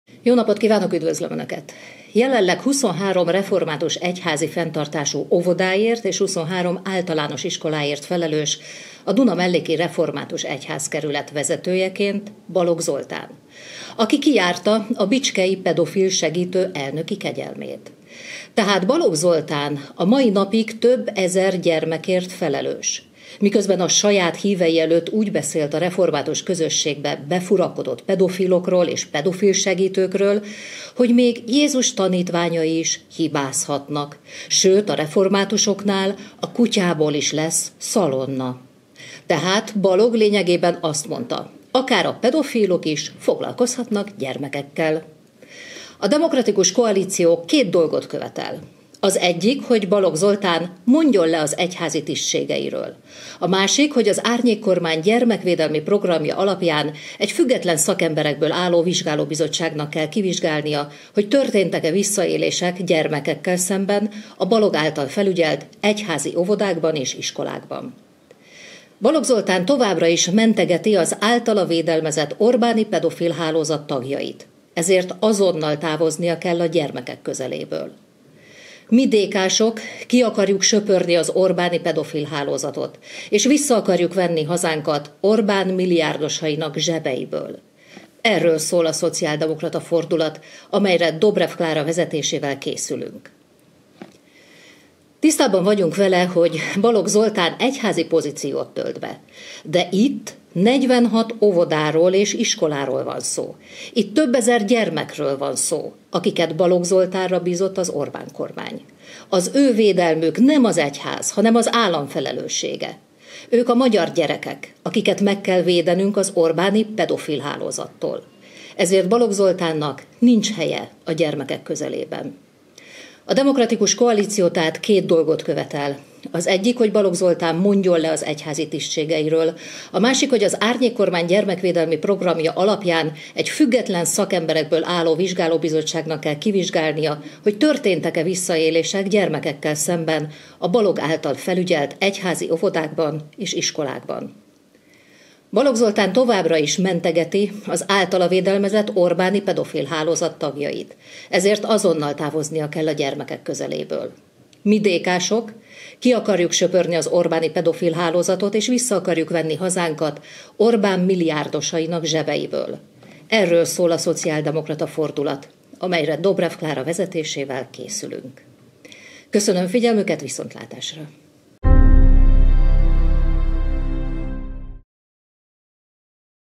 Negyvenhat óvoda és iskola tartozik Balog Zoltán püspöksége alá!
Balog Zoltán a mai napig több ezer gyermekért felelős, miközben továbbra is mentegeti az általa védelmezett orbáni pedofilhálózat tagjait.

Balog Zoltán nem maradhat tovább püspök. Le kell mondania és vizsgálatot kell indítani az általa felügyelt intézményekben!

Kövesd élőben, és oszd meg Kálmán Olga, az Árnyékkormány szóvivőjének sajtótájékoztatóját!